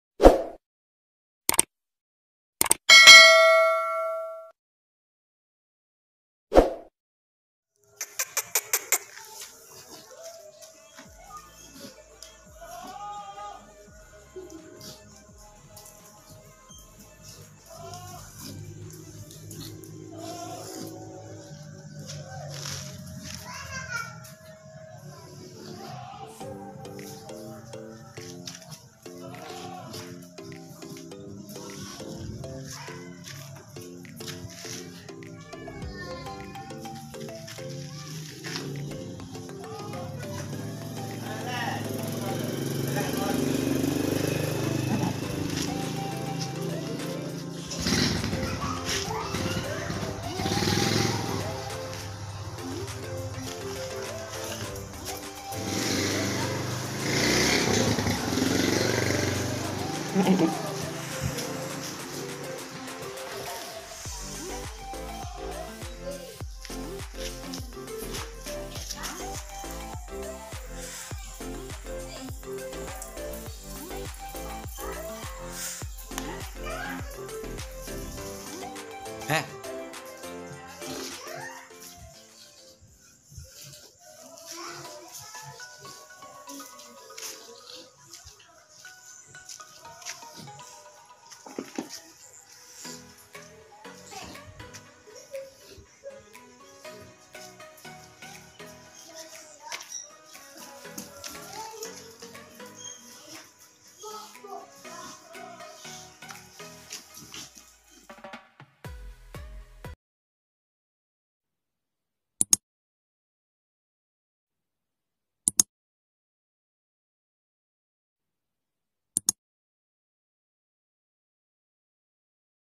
Mantul|Etawa super terbesar dan terjumbo
pejantan super jumbo super besar
pe ras kligesing
tb 1 meter

ikuti terus video kami jangan lupa
KOMEN
DAN SHARE
TERIMAKASIH

WA.082281990419